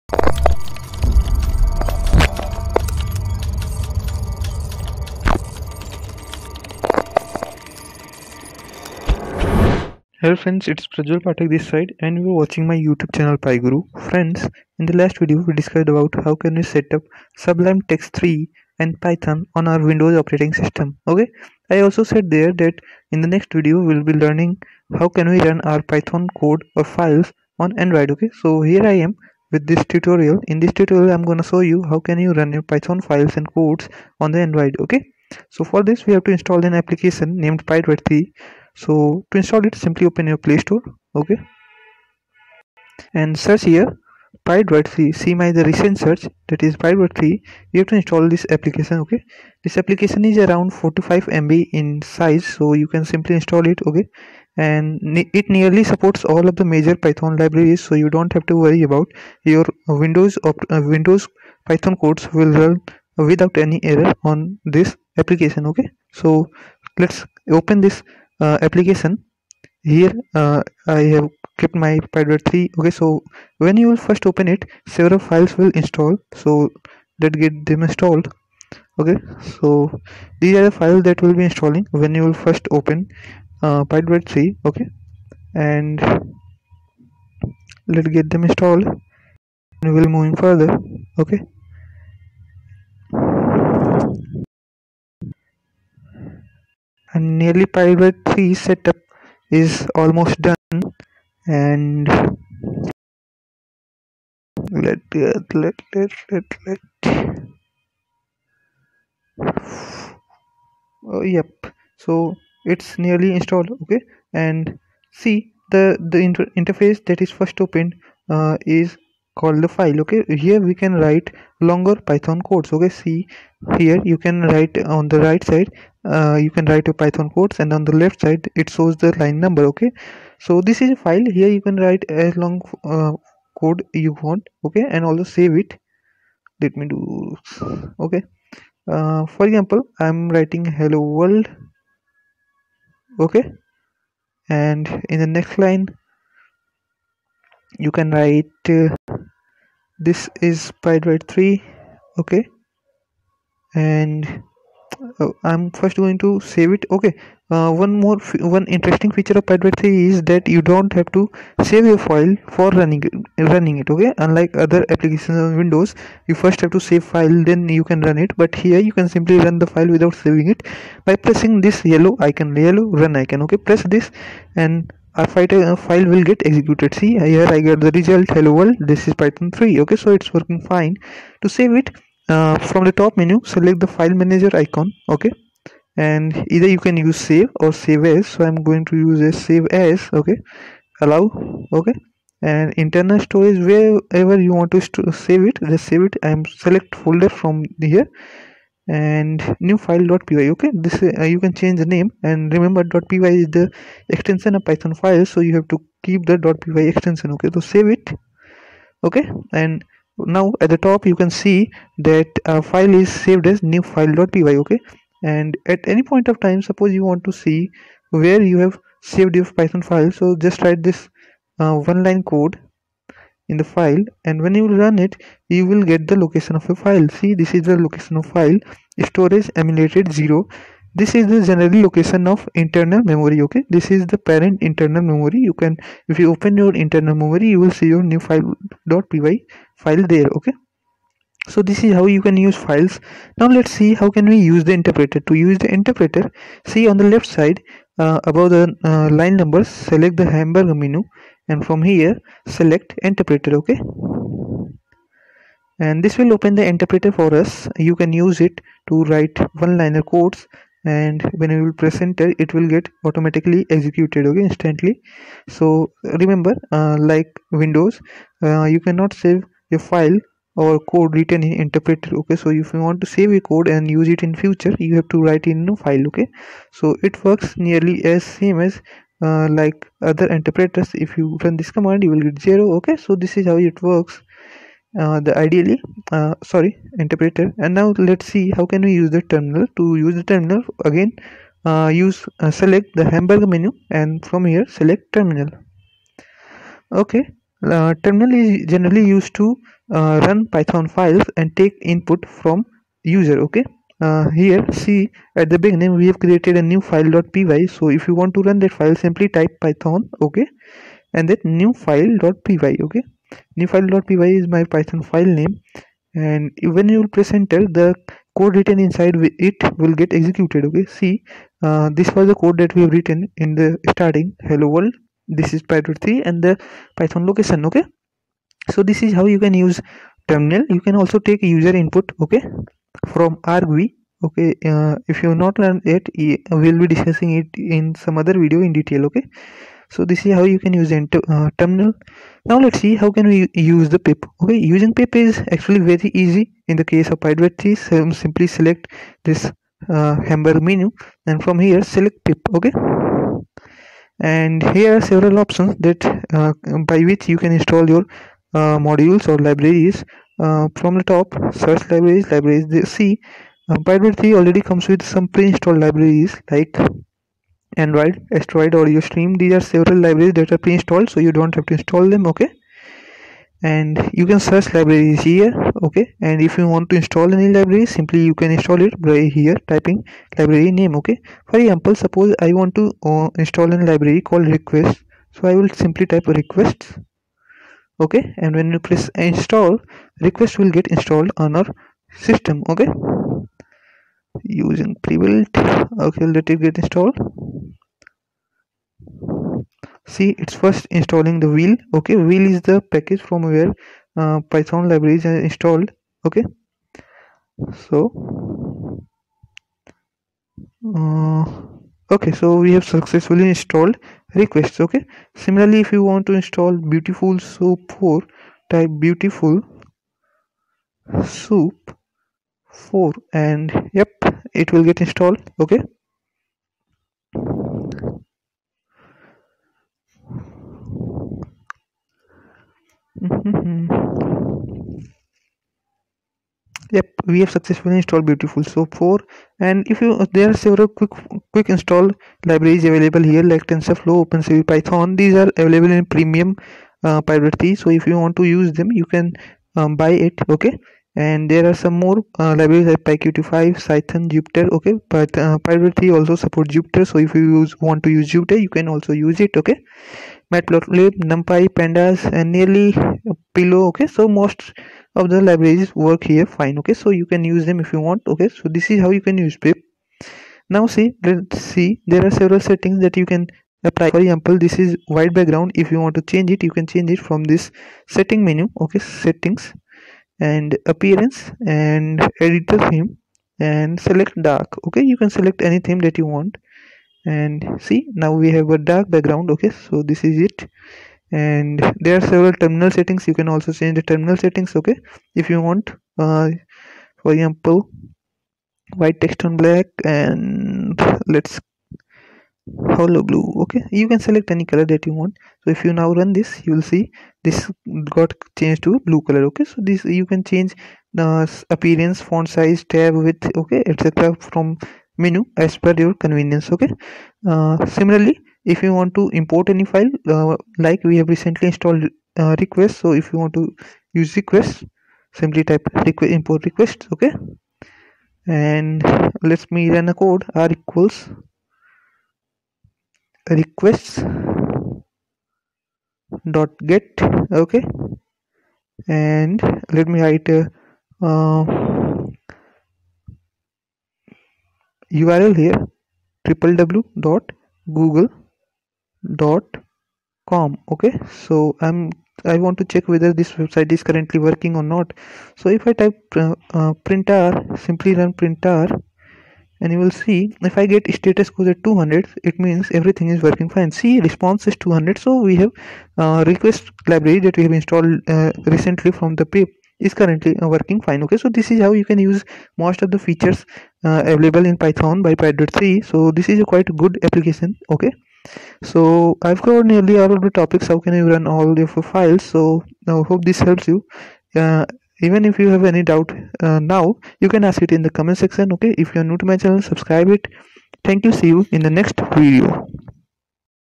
pyTalk 4 | Running Python on android with pydroid 3
Hello YouTube,
      In this video we'll be talking about how can we run our Python codes on android. We'll also be seeing :
* How to create, save and run files
* How to use shell /
* How to use terminal
* How to use
* How to install modules/libraries using pip
* How to change terminal settings
* How to change theme in pydroid3

Download free programming ebooks on more than 45+ programming languages

pythonguru python guru